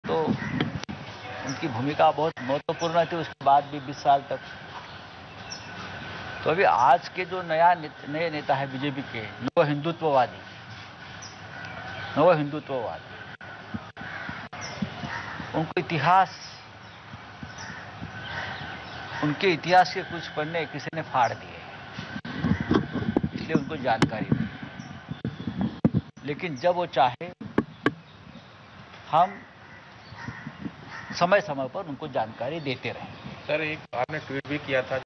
Sanjay Raut : भाजपचे नवे नेते नवहिंदुत्ववादी, त्यांना इतिहास माहित नाही | Shivsena - BJP
ABP Majha LIVE: Maharashtra Coronavirus Restrictions 2022 | मराठी बातम्या | COVID19 | LIVE TV 24x7